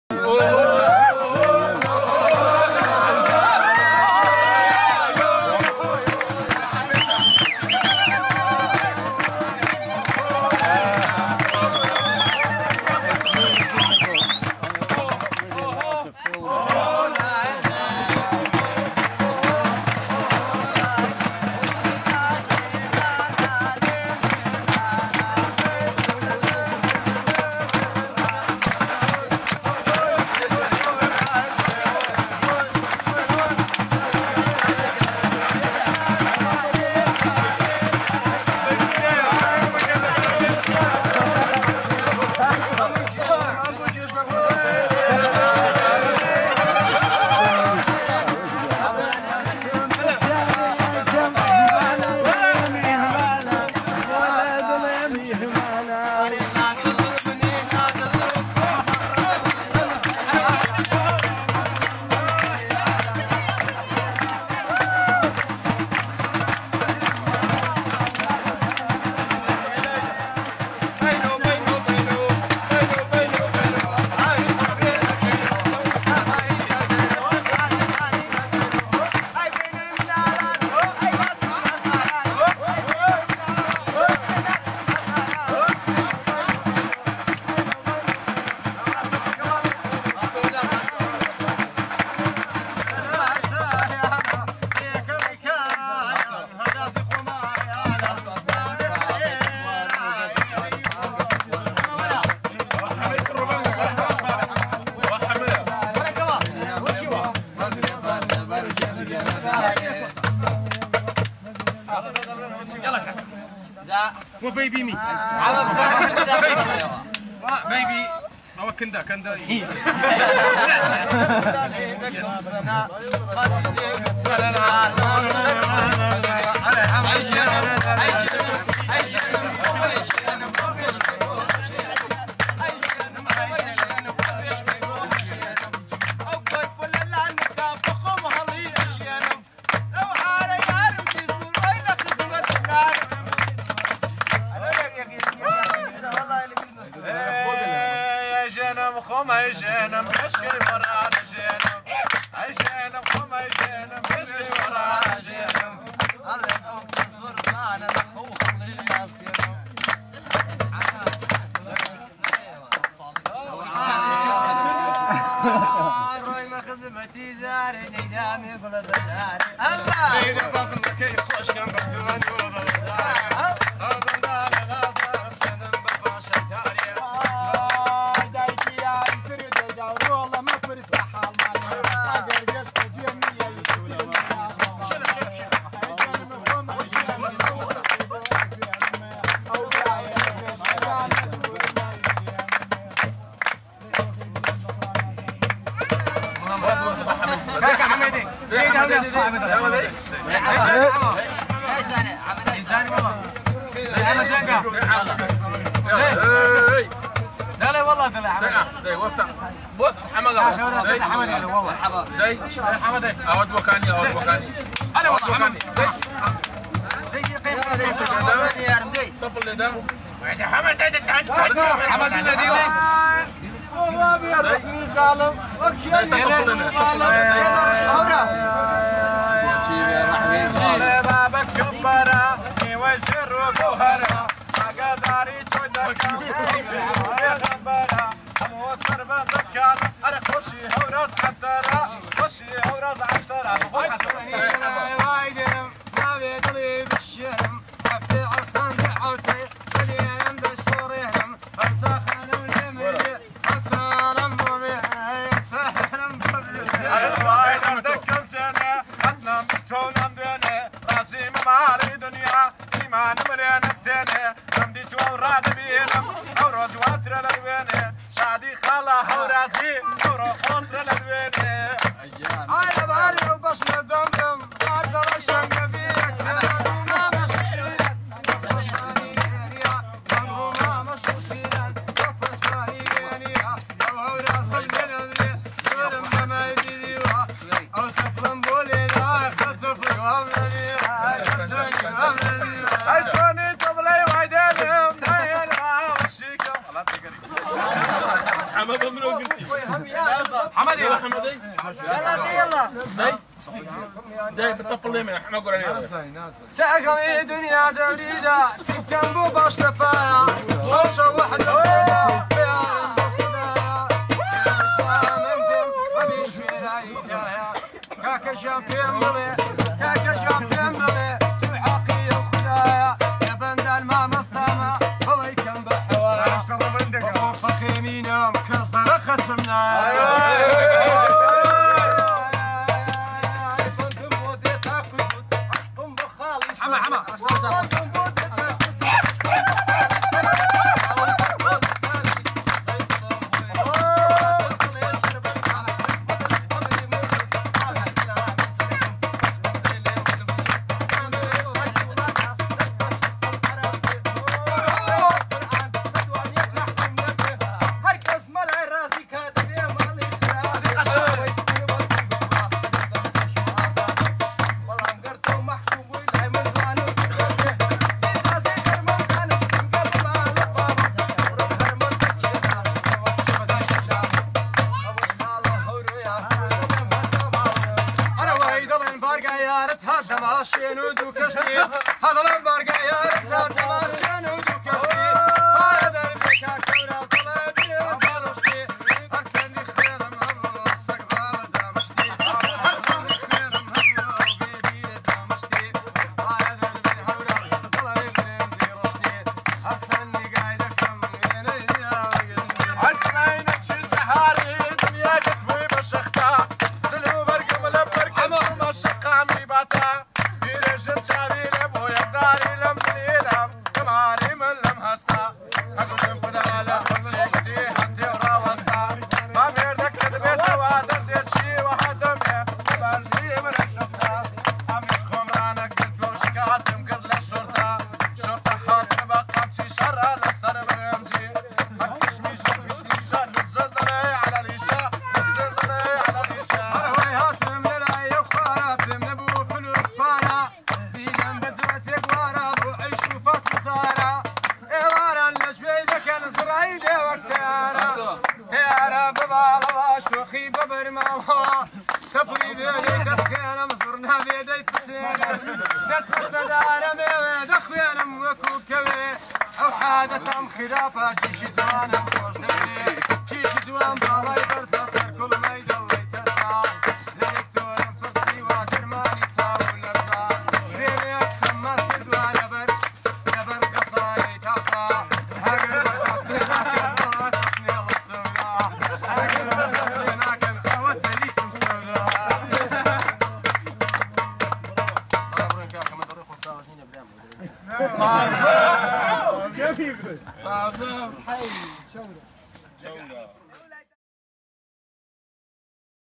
Kurdish singers - Improvised poetry - Dunkerque refugee camp - 12 December 2016
We travelled to Grande Synthe near Dunkerque, Northern France,  in a journey of musical solidarity with the refugees and migrants gathered in that town in the hopes of reaching England.
The population of the camp numbers 1,050 - not counting the very many children in the camp. The great majority of the people here are Kurdish, and most of them are from northern Iraq (Kurdish Regional Government). At this time a major war offensive is taking place there, to regain the town of Mosul from the Islamic forces of IS / Daesh.
On Monday 12 December we did a special concert for the women and children in the Women's Centre.
Then we went to the tea tent, taking Kurdish drums with us.
The drums made the moment, as they always do. Passing around from hand to hand. People trying different rhythms, different styles, matching them with snatches of songs, seeking to catch the fire that comes when the groove is right. Sometimes the rhythms are Arabic, sometimes they are Kurdish.
Eventually the word goes round the camp, and the singers come and join the company gathered around the wood-burning stove.
Many songs are sung. Then, at a certain moment, the drumming falls into the familiar rhythm that accompanies the singing of improvised verses. The men trade verses between them, and the drumming is accompanied by clapping, laughter, whistling and shouting.
A rare and precious moment in this alien, hostile and racist terrain.